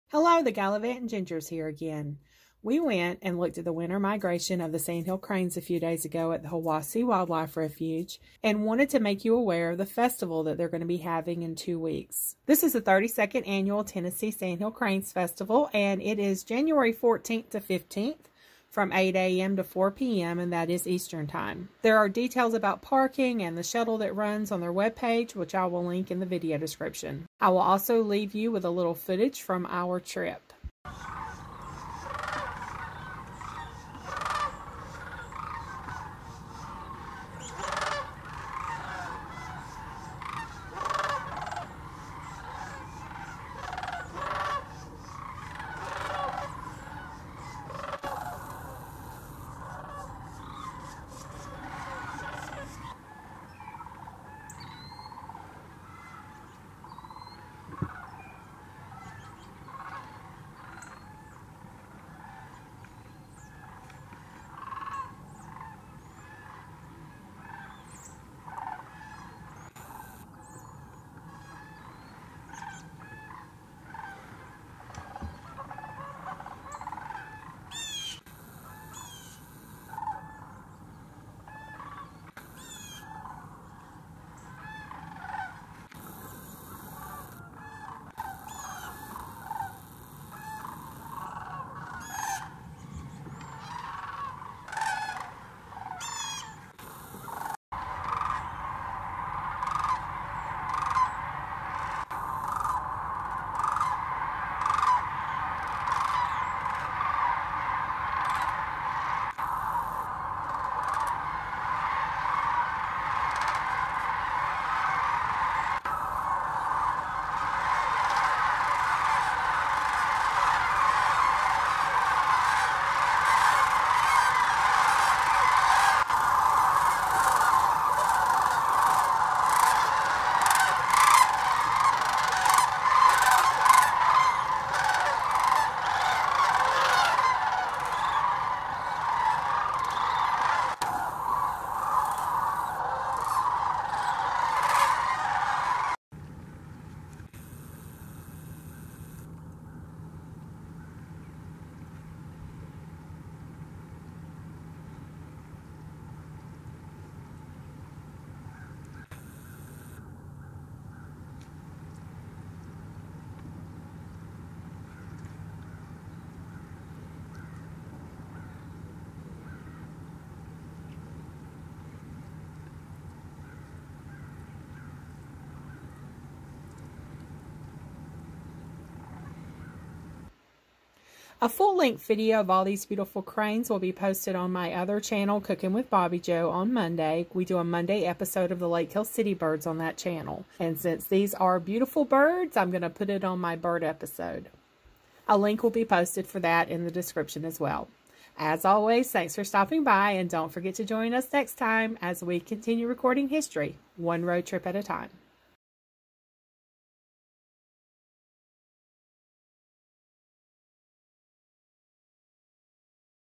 32nd Annual Tennessee Sandhill Cranes Festival
Today we travel to Birchwood, TN to the Hiwassee Wildlife Refuge to see the Sandhill Cranes. We saw thousands of these beautiful birds. On Jan 14 - 15 they are having the 32nd annual Sandhill Crane Festival.
I also have a blog post about these birds with photos.